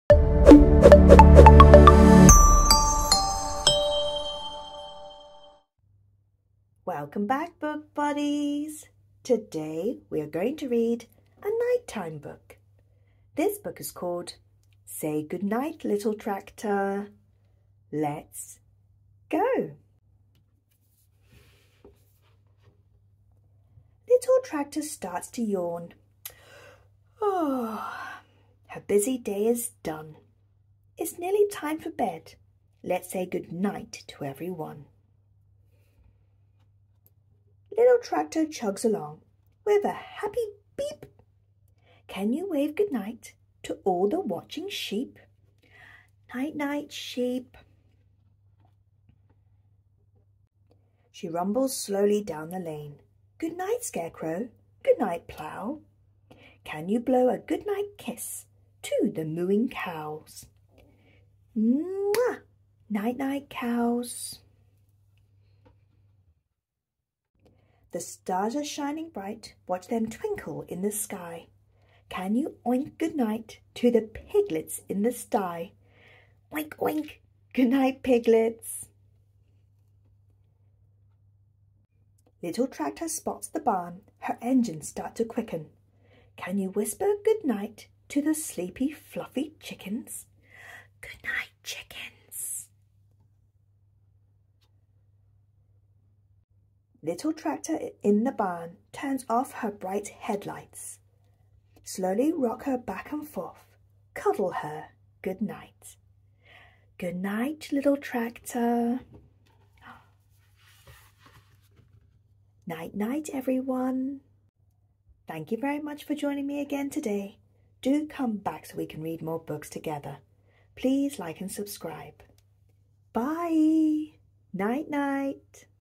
Let's read together Say Goodnight, Little Tractor. Read along. Children's bedtime stories.
Let's read together Say Goodnight, Little Tractor

Help Little Tractor to say goodnight to the farm in this gentle, interactive bedtime board book.

This gentle, rhyming story follows Little Tractor as he chugs through the farm and travels home to bed. Along the way, the reader is invited to say, sing or kiss goodnight to all the creatures on the journey.

This peaceful, snuggly bedtime story is ideal for sharing with your toddler and for building bonds. As the sun sets, the soft scenes become more bedtime-friendly and as you say "goodnight" to the farm, toddlers also wind down and get ready for bed themselves.

A perfect first bedtime book for toddlers who love to say goodnight!

Bedtime stories
Read along out loud
Read along together aloud

Say Goodnight, Little Tractor: Join in with this sleepy story for toddlers